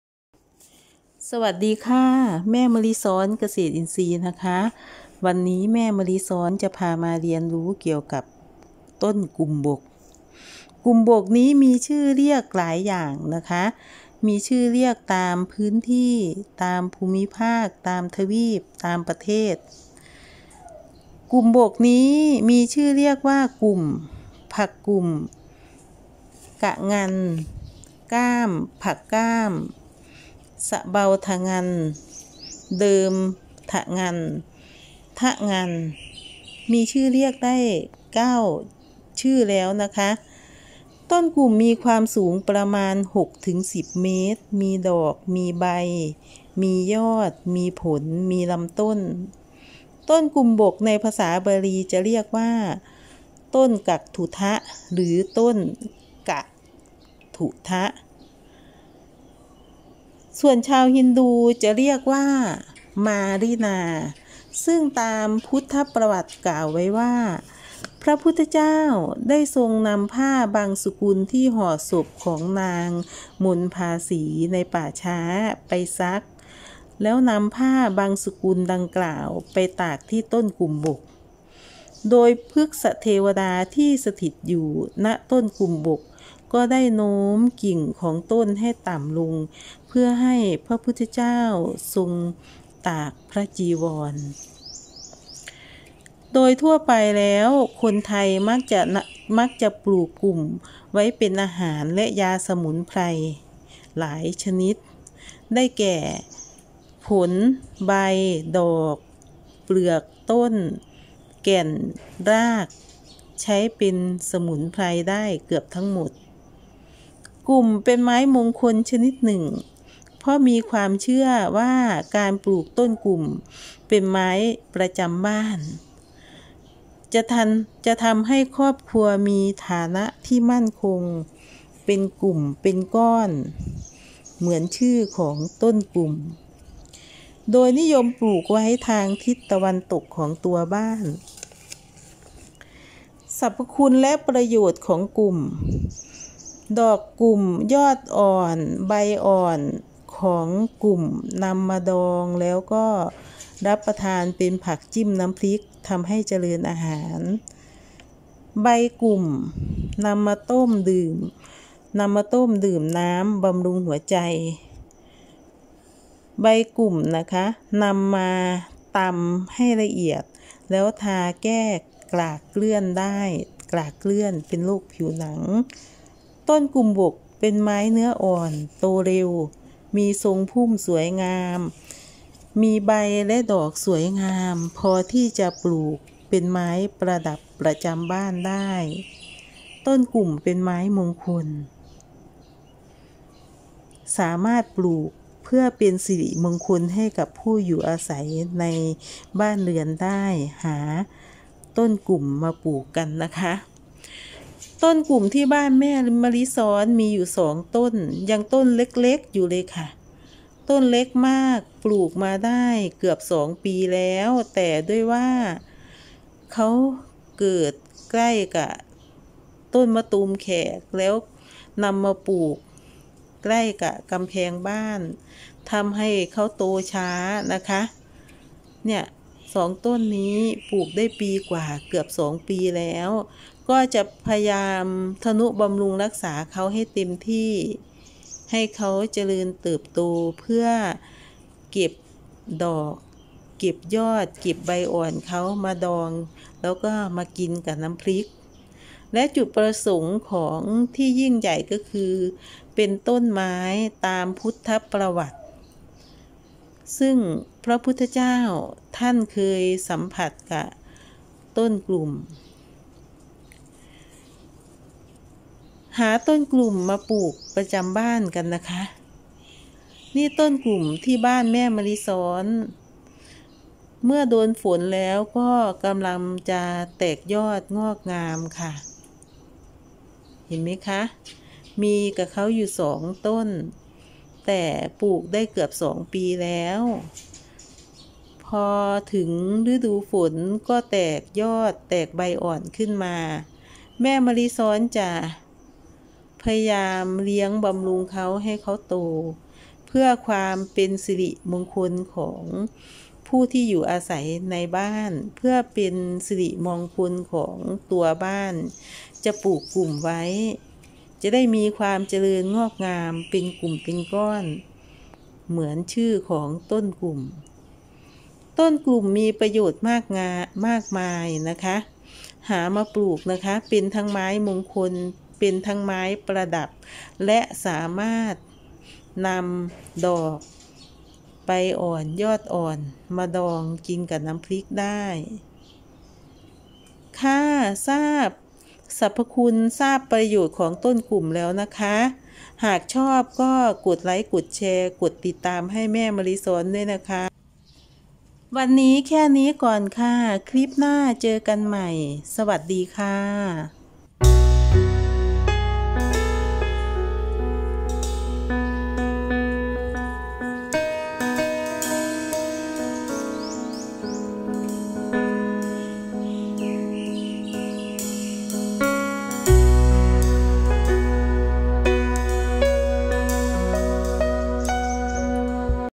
ผักกุ่ม​ กุ่มดอง​ ผักกุ่มกินได้และเป็นไม้มงคลมีคำกล่าวเกี่ยวข้องกับพุทธประวัติ
ผักกุ่ม​ ผักกุ่ม​ดอง กุ่มบก​ กุ่มเป็นไม้มงคล​ นอกจากนี้ใบอ่อน​ ยอดอ่อน​ ดอก​ ของกุ่มนำมาดองกินกับน้ำพริกได้แม่มะลิซ้อนเกษตรอินทรีย์
Fanpag: แม่มะลิซ้อนเกษตรอินทรีย์
โทร.093-8943601​